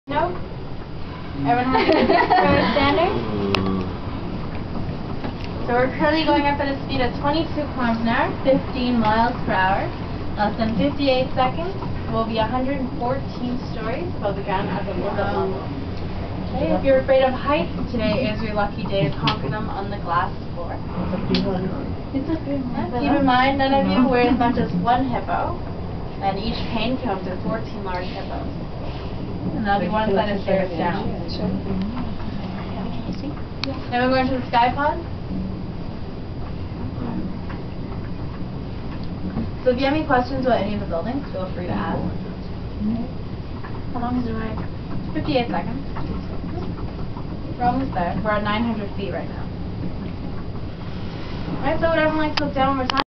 CN Tower elevator ride
On the elevator ride up the CN Tower in Toronto, Canada